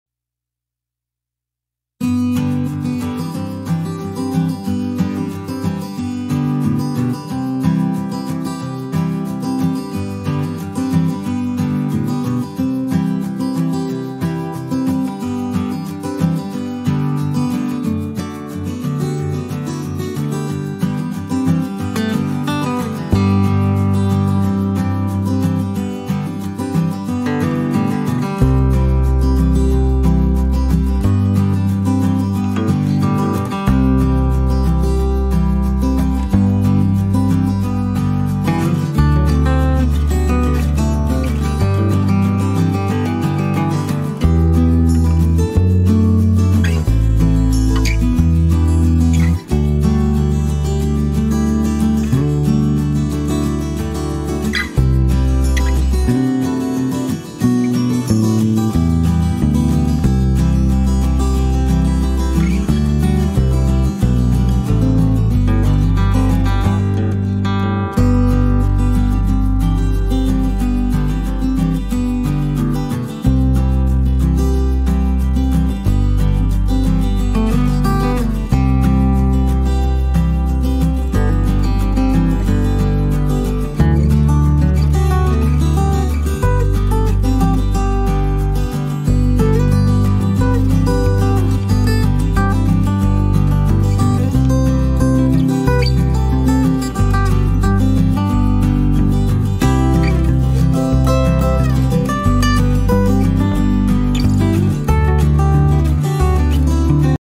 How To Solve IndianOil ONE App Keeps Stopping Problem || Rsha26 Solutions
How To Solve IndianOil ONE App Keeps Stopping Problem

Hii friends welcome to my YouTube channel Rsha26 Solutions, today I am showing you how to solve IndianOil ONE app keeps stopping problem through this video hope so this will be helpful for you if you are facing the same issue.